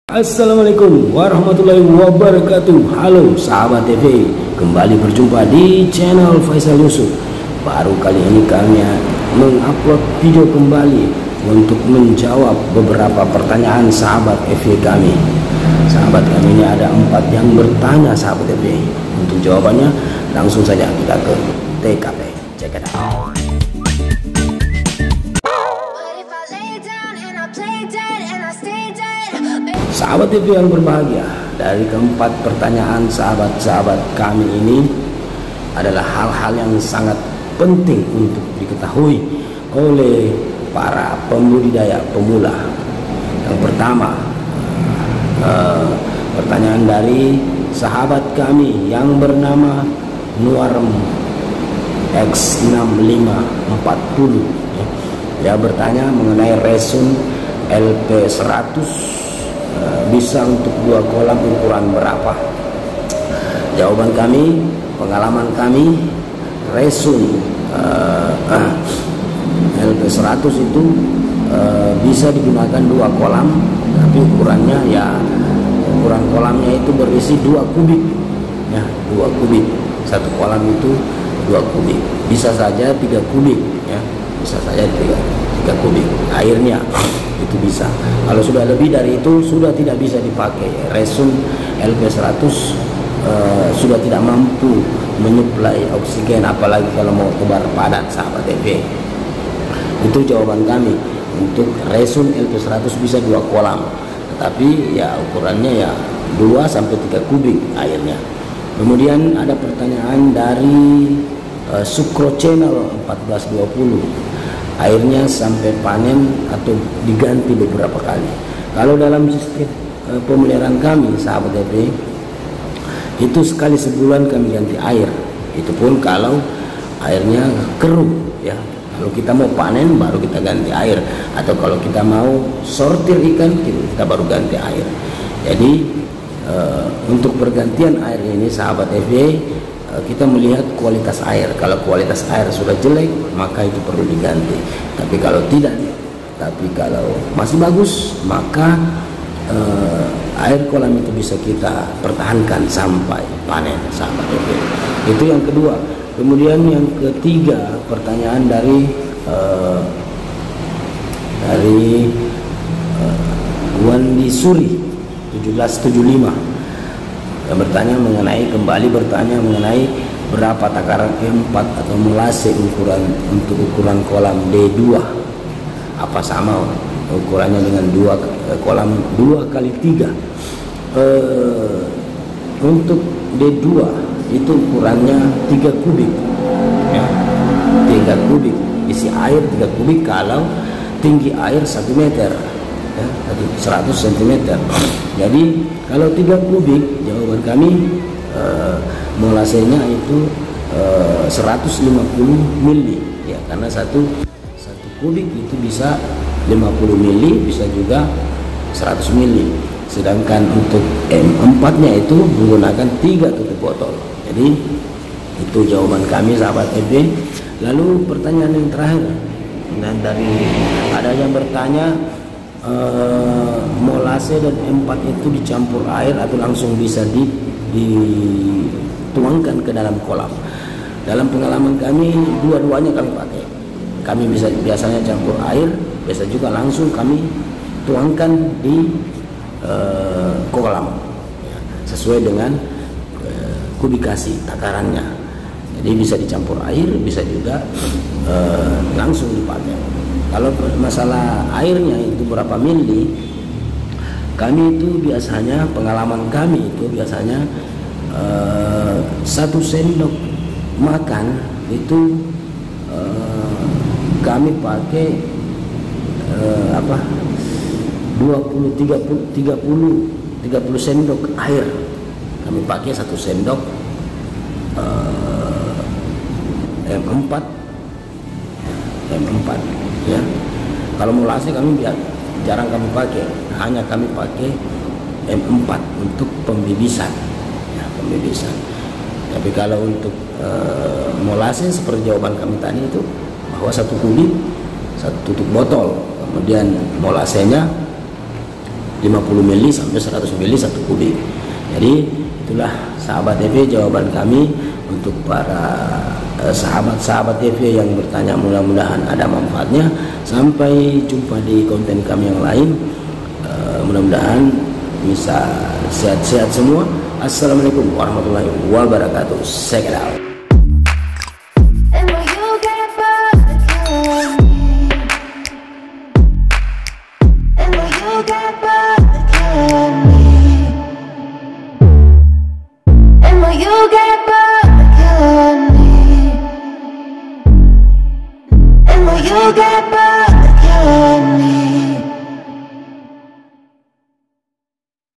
PENTING, Untuk Di Ketahui Oleh Pembudidaya Ikan Nila PEMULA
Assalamualaikum video kali ini kita akan berbagi pengalaman tentang seputar hal yang PENTING, Untuk Di Ketahui Oleh Pembudidaya Ikan Nila PEMULA, yaitu mengenai kwalitas air kolam, aerator kolam ikan nila, molase dan em4.